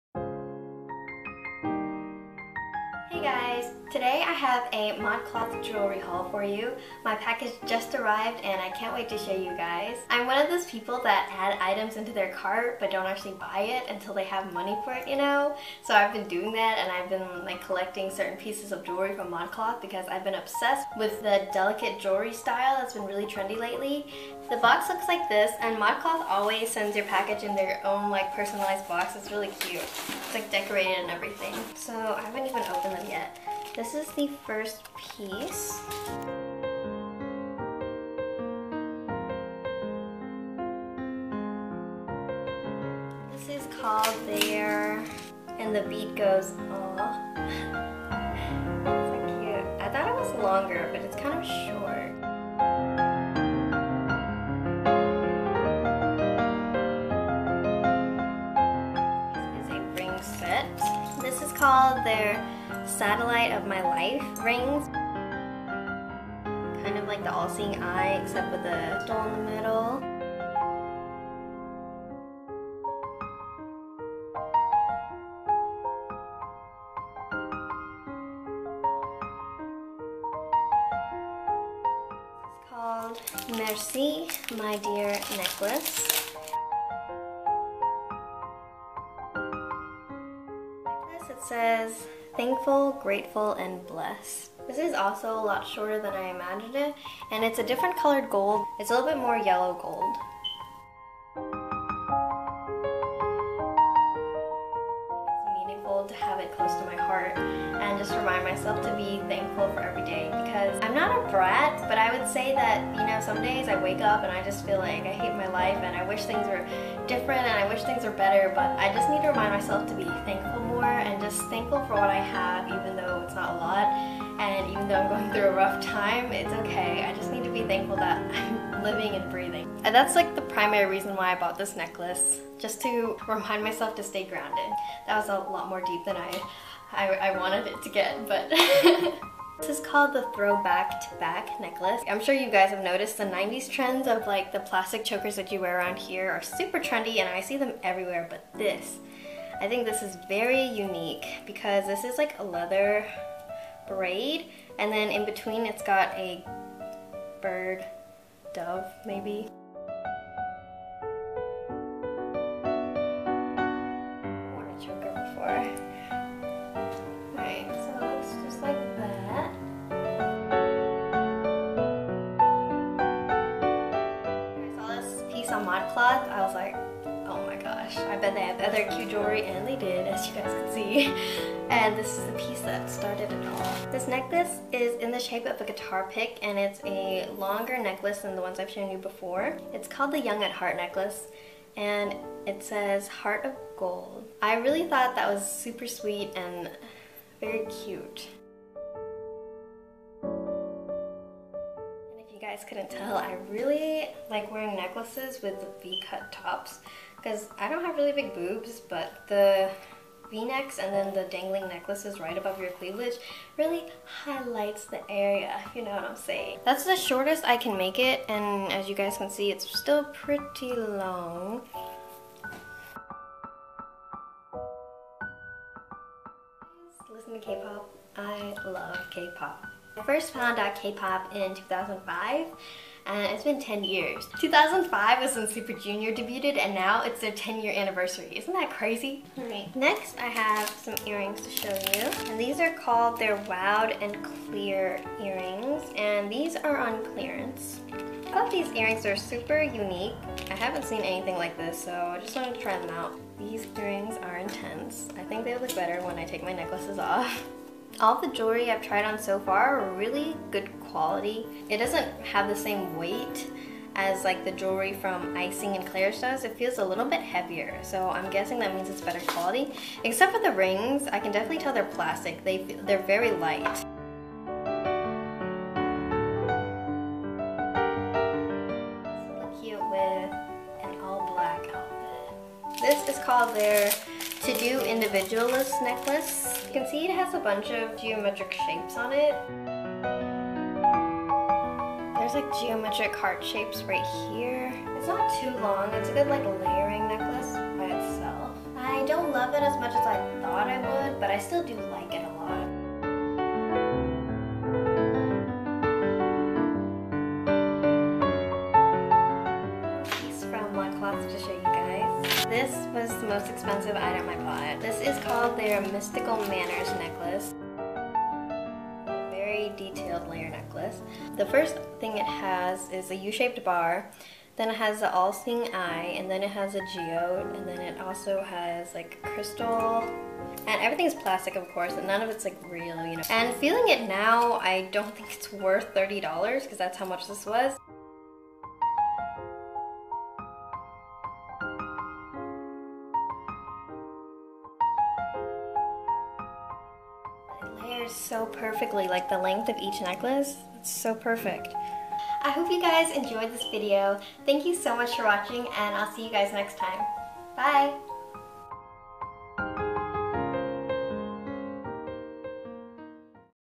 ModCloth Jewelry Haul
I've been obsessed with the delicate jewelry trend and layering necklaces! I also love KPOP a lot.

➤1st Tattoo Experience // Pain Level, Cost, & Advice!!!